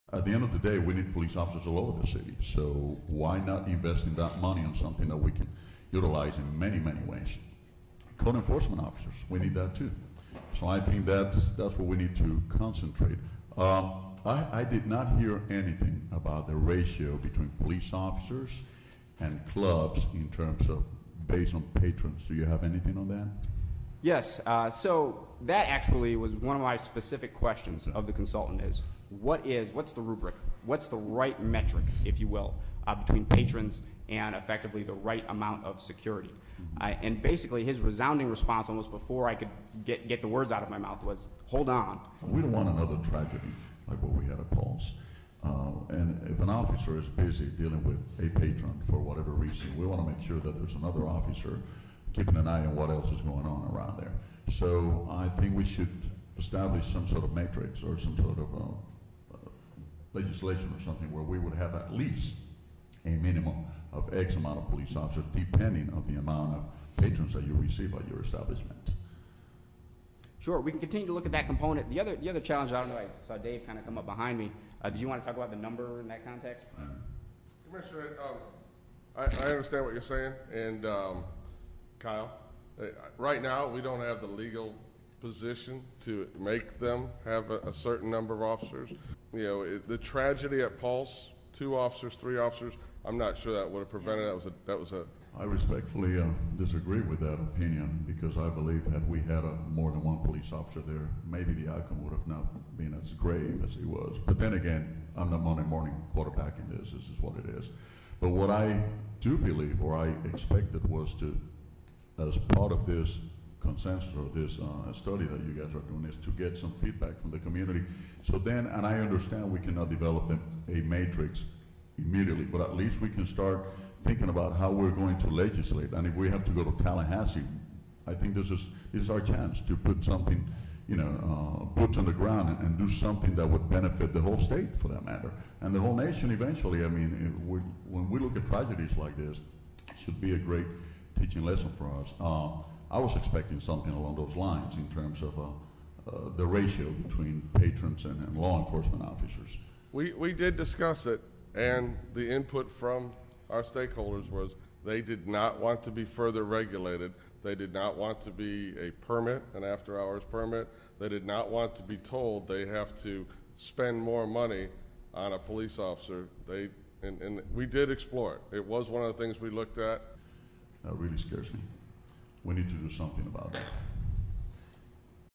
Orlando City Councilman Tony Ortiz Talks Security Negligence at Pulse Nightclub During Workshop
Tony Ortiz of the City of Orlando City Council goes against David Arnott's assessment of the security situation at the Pulse Nightclub during the mass shooting that occurred on June 12, 2016.

During this November 14, 2016 City Council workshop, Mr. Ortiz wanted to know what can be done to make sure there was enough security at nightclubs so that a gunman cannot enter the premise without being checked at the front door. This is what happened at Pulse.

David Arnott, who works in the Office of the Mayor (Buddy Dyer), is on record lying about Pulse nightclub. Particularly about the narrow exit route that was littered with trash cans on the night of the shooting when he was asked about this during a Nightclub Task Force Meeting by an Orlando Sentinel reporter. Both Arnott and Mayor Buddy Dyer also work directly with the Pulse Nightclub owner's nonprofit, the OnePULSE Foundation.

"Code enforcement officers... we need that too!"

"We don't another tragedy like what we had at Pulse and if an officer is busy dealing with a patron for whatever reason we want to make sure that there is another officer keeping an eye on whatever is going on around there."

"I respectfully disagree with that opinion, because I believe that if we had more than one police officer there maybe the outcome would not have been as grave as it was..."